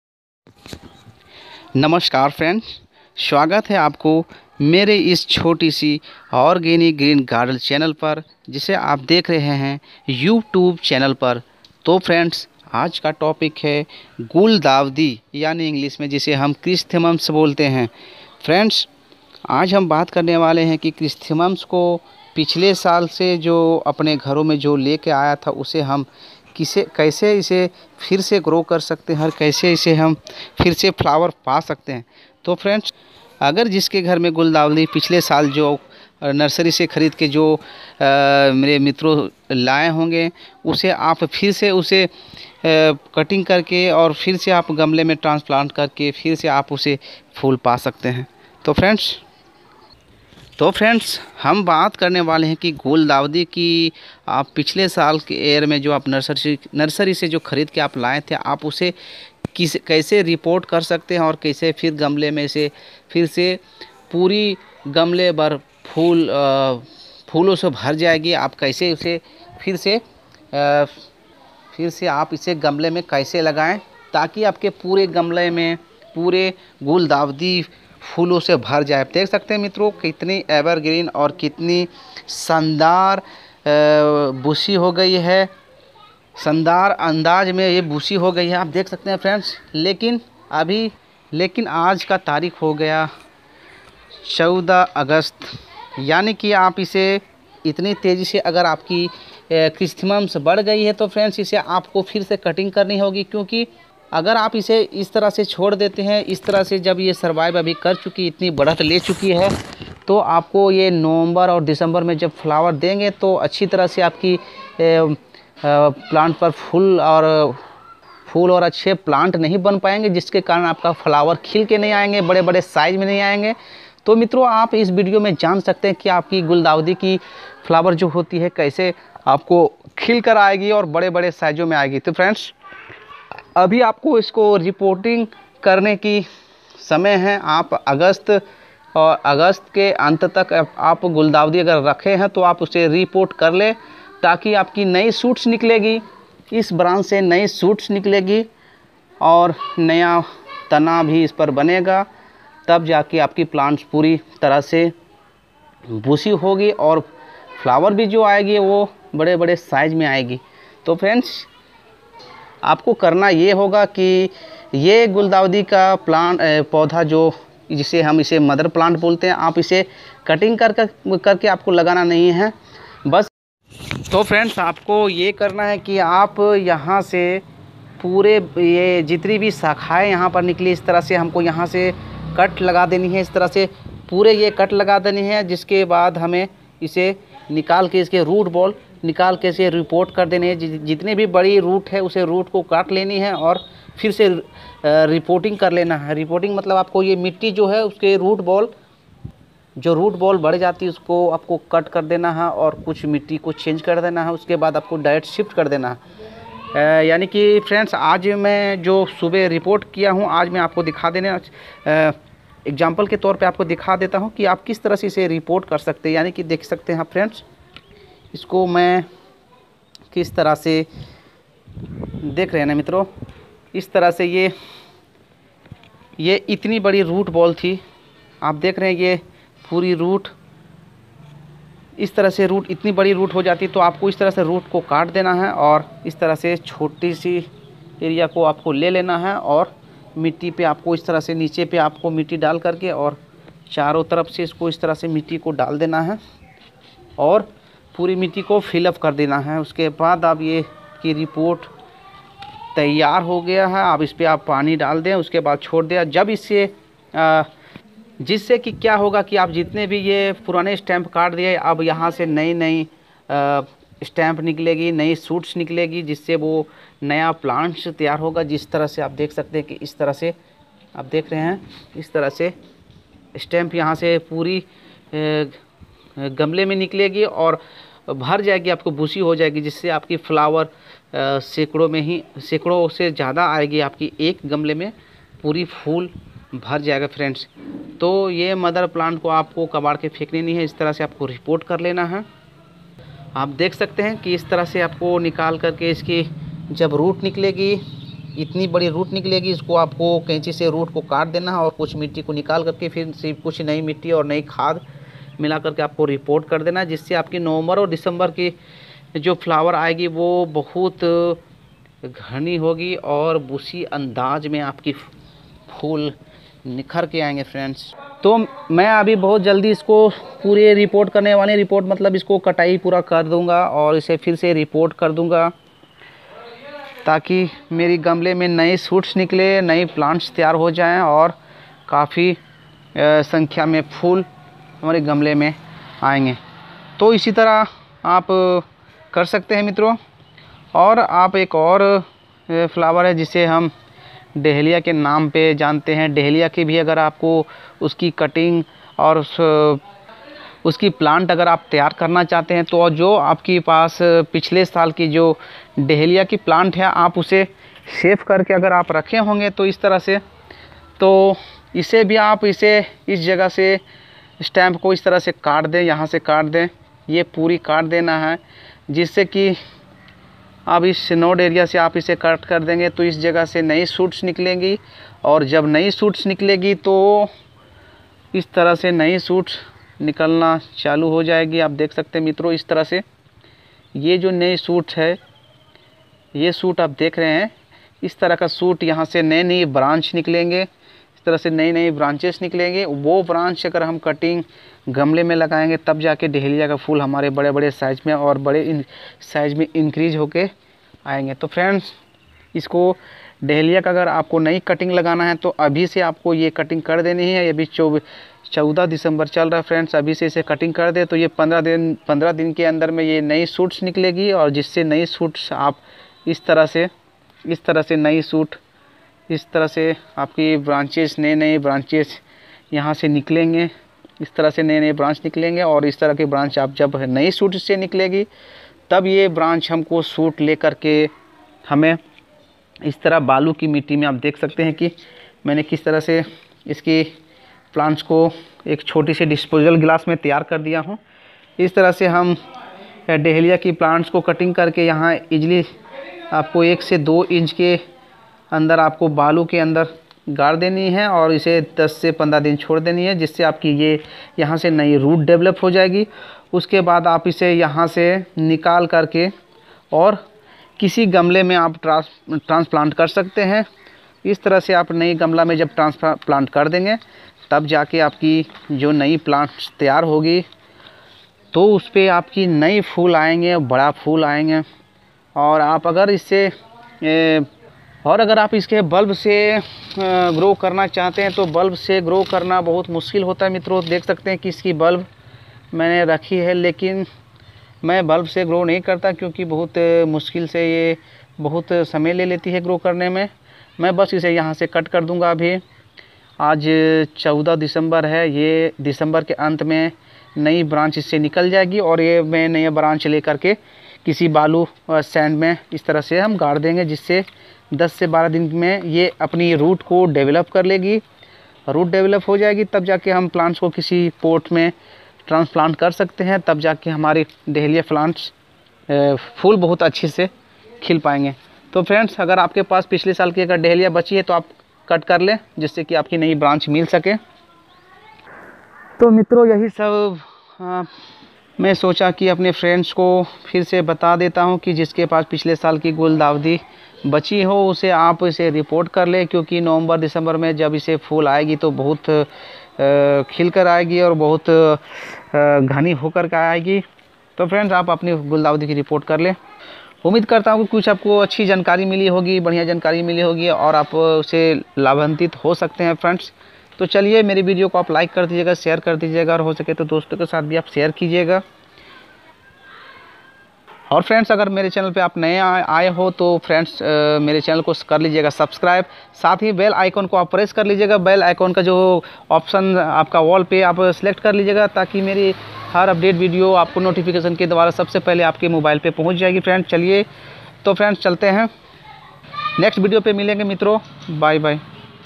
गुलदावदी के फूल मात्र 2 रू में आपके घर पर guldawdi guldawadi ki report || गुलदावदी पौधे के
प्यारे दोस्तों --...guldawdi ke phool apke ghar par pichle shaal ke ho to ap es bidhi se ap agle shaal bhi phool pa sakte hai jase ki video me bataya gaya hai

my chhenal organic green garden